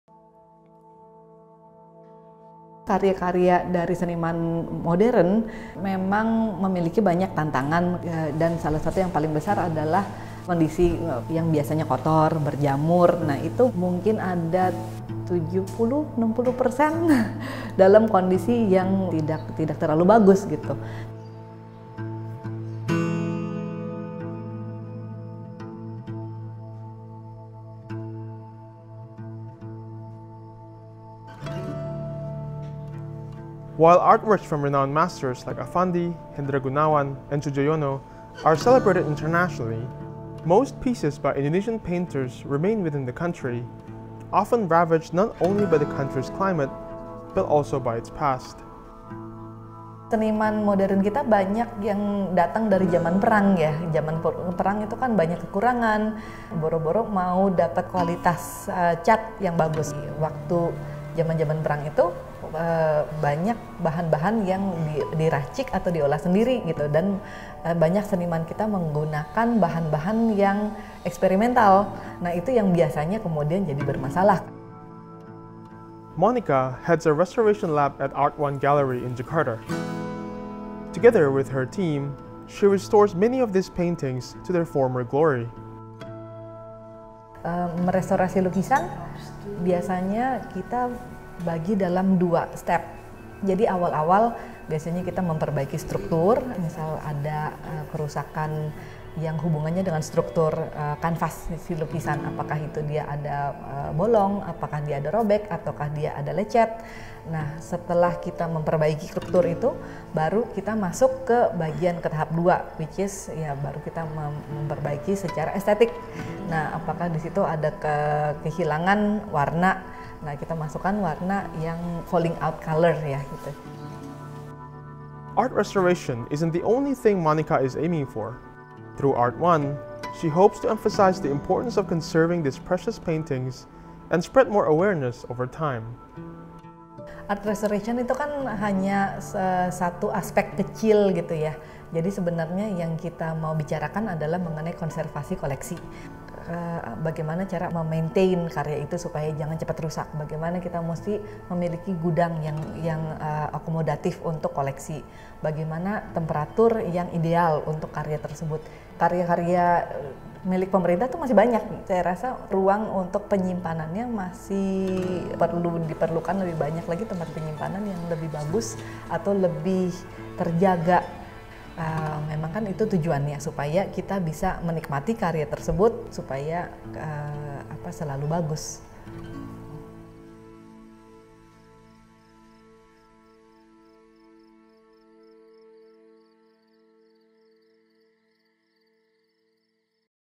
INDONESIA: Art of restoration
Withering works of art are being carefully restored at this studio in Jakarta, where paintings by celebrated Indonesian artists get a facelift.